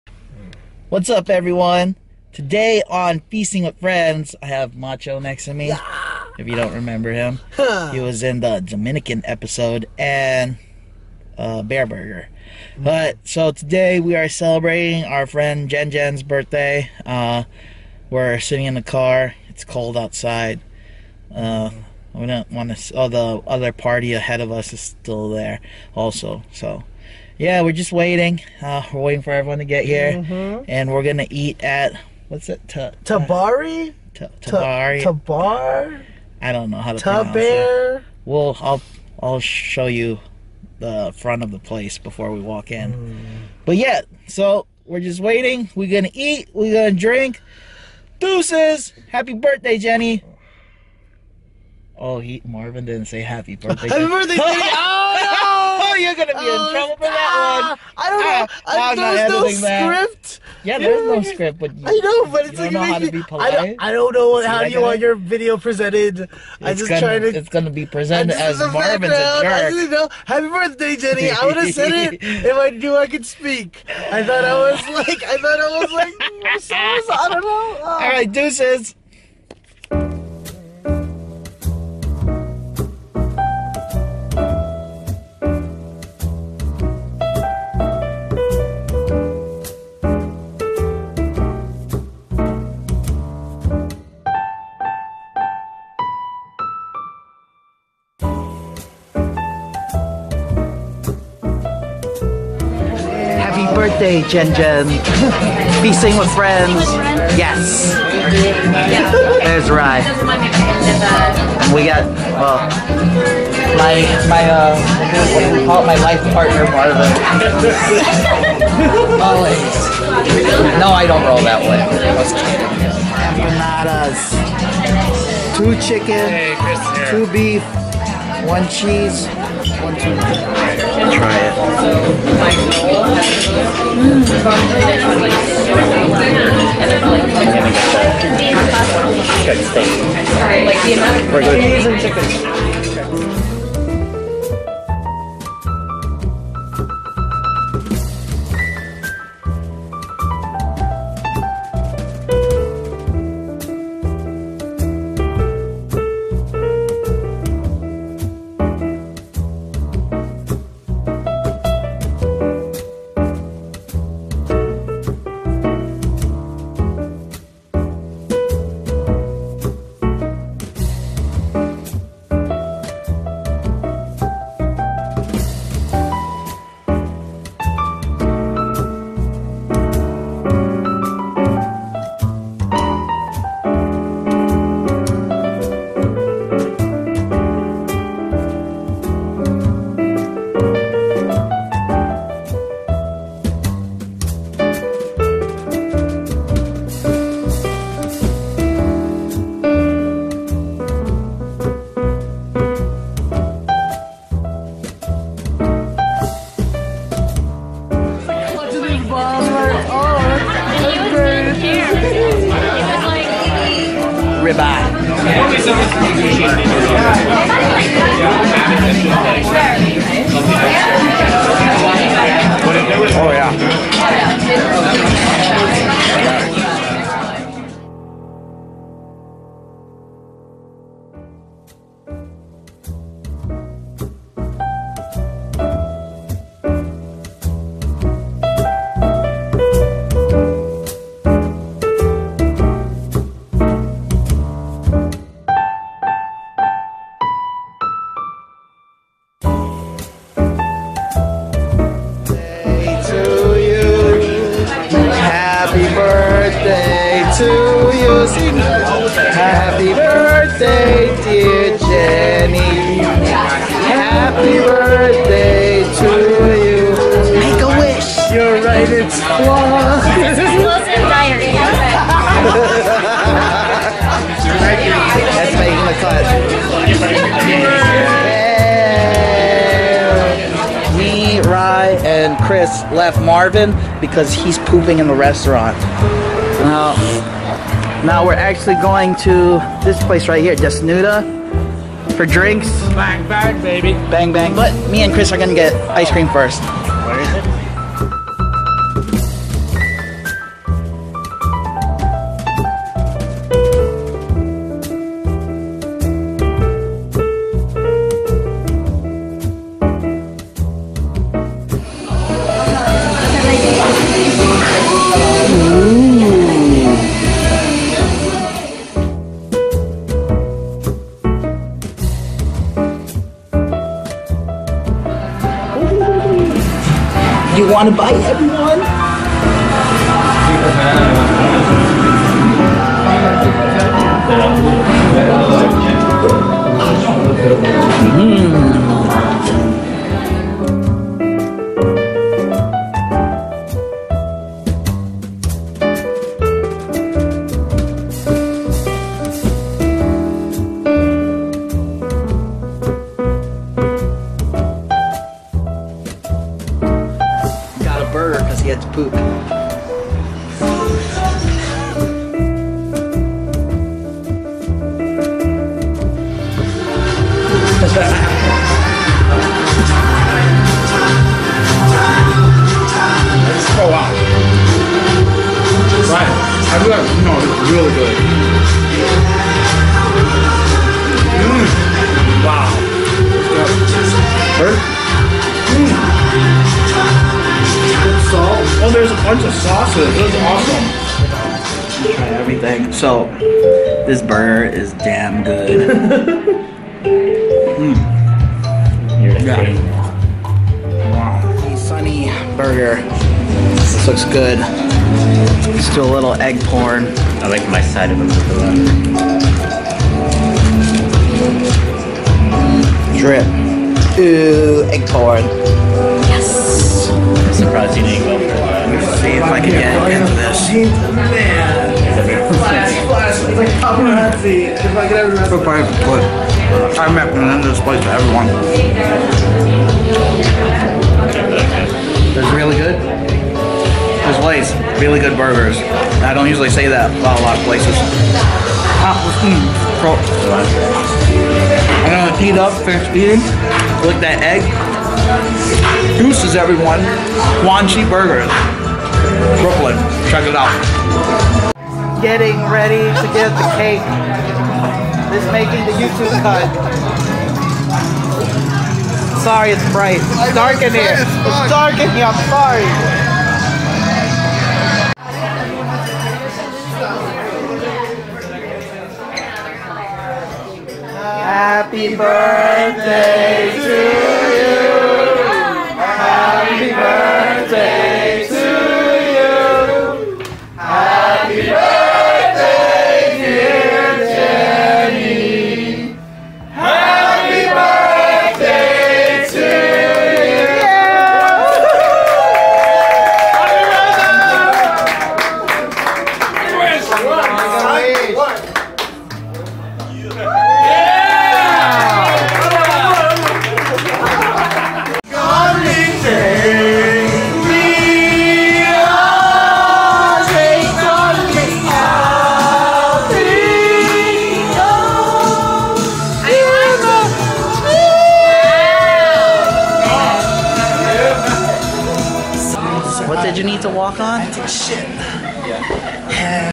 FwF Ep. 21 Tabaré, -321° Ice Cream Shop, and Juanchi’s Burger Happy Birthday Jenny!
in this episode of Feasting with friends we are celebrating one of my best friends (Jenny) birthday. . Then pulled a bang bang and went . Because Marvin had to poop again we stopped again for another bang bang bang and went where I loved the burgers. Definitely a recommend in my books!